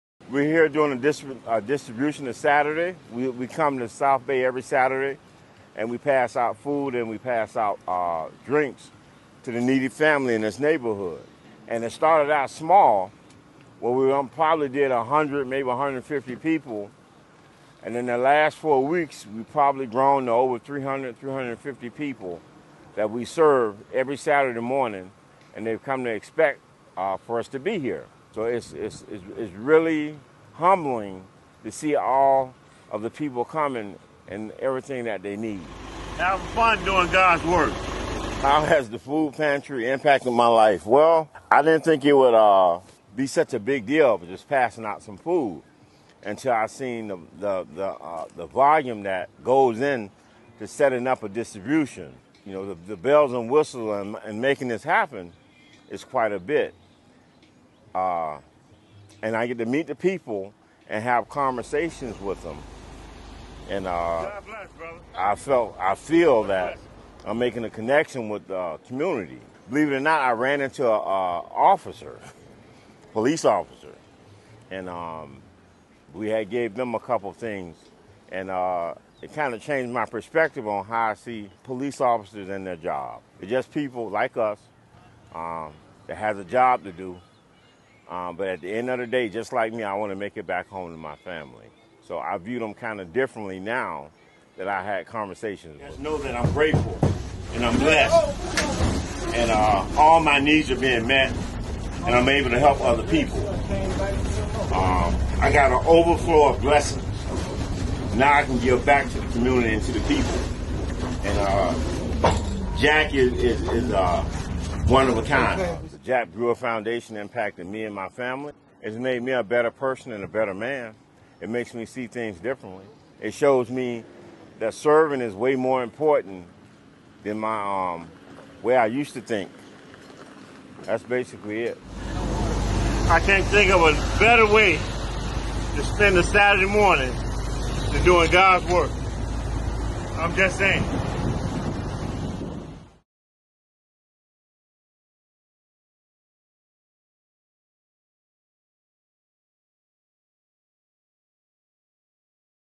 Leon has transitioned from prison to become a true servant of God
In this inspiring video, witness Leon’s incredible transformation from prison to becoming a true servant of God. With the support and guidance of the Jack Brewer Foundation, Leon has found faith, purpose, and a renewed sense of direction in his life. Hear his powerful testimony of redemption, resilience, and dedication to serving others. Join us to celebrate Leon’s journey and see how faith can lead to profound personal change.